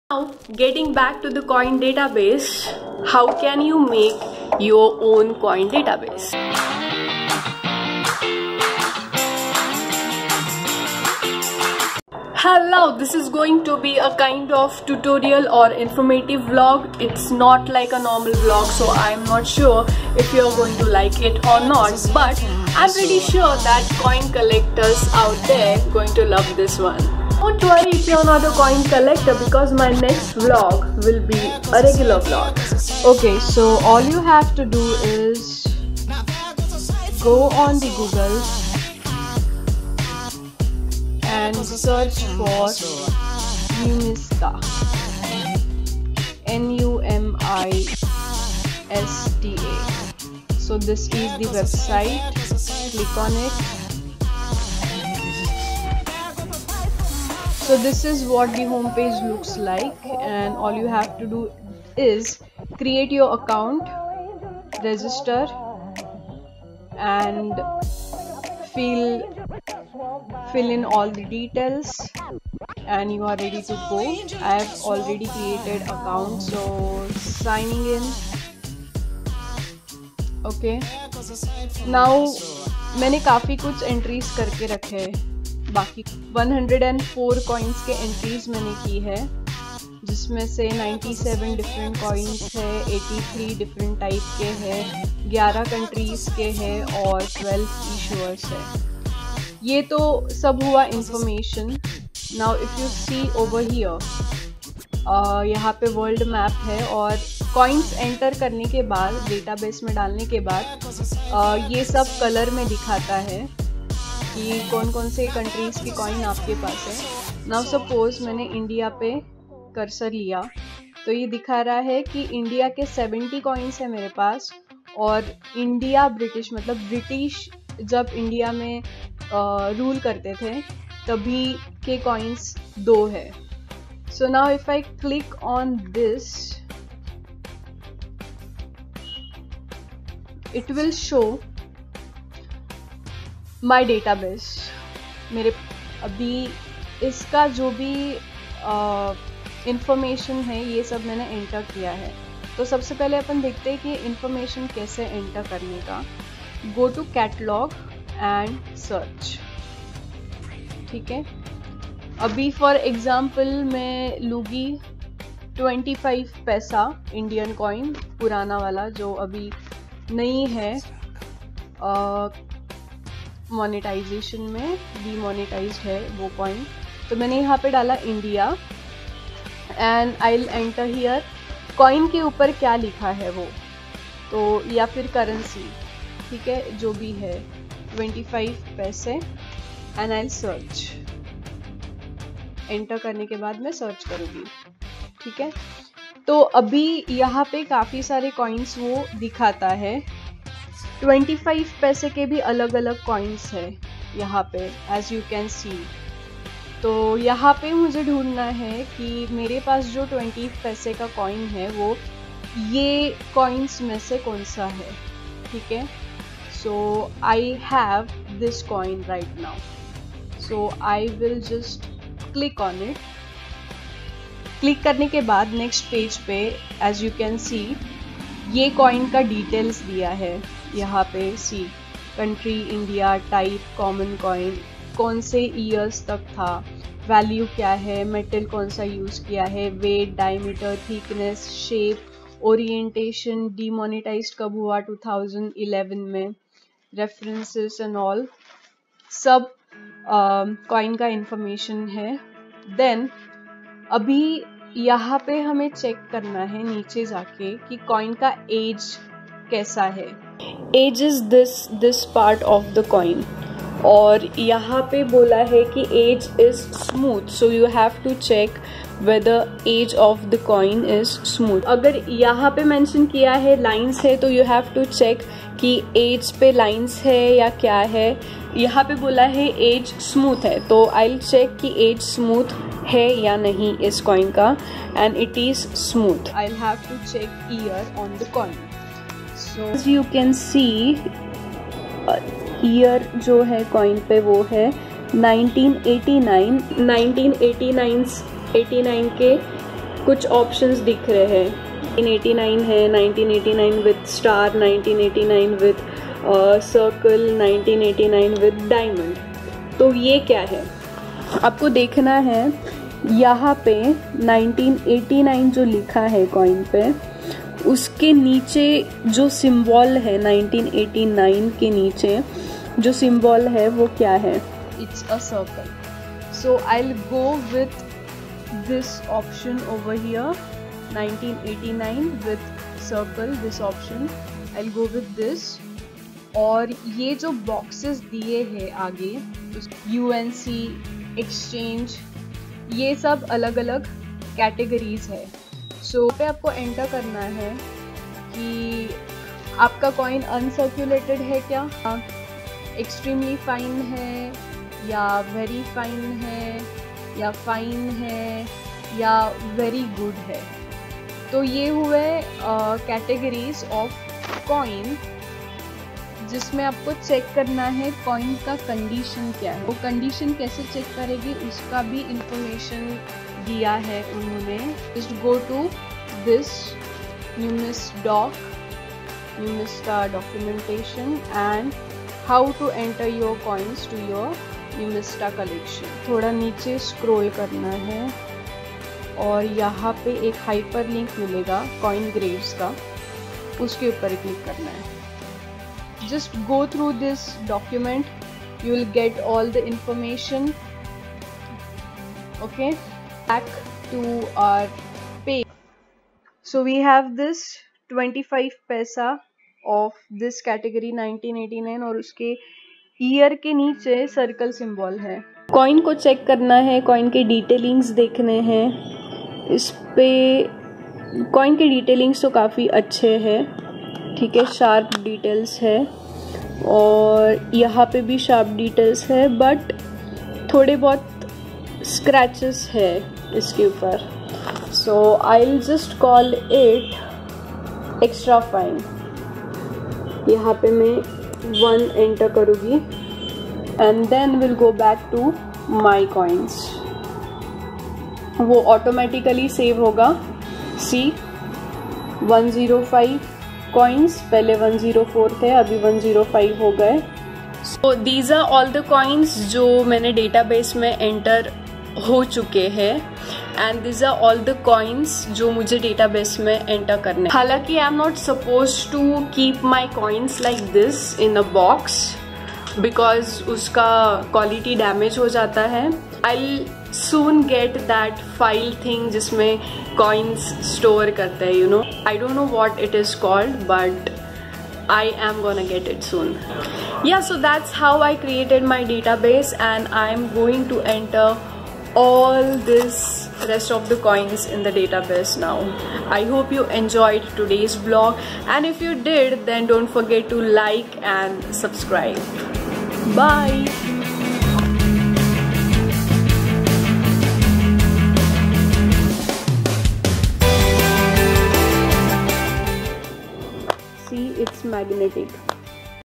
How to make a Coin Database | Vlog#15
This is how I made my coin database. Its a fun website though. Also you will get a lot of information that you may not know through the same.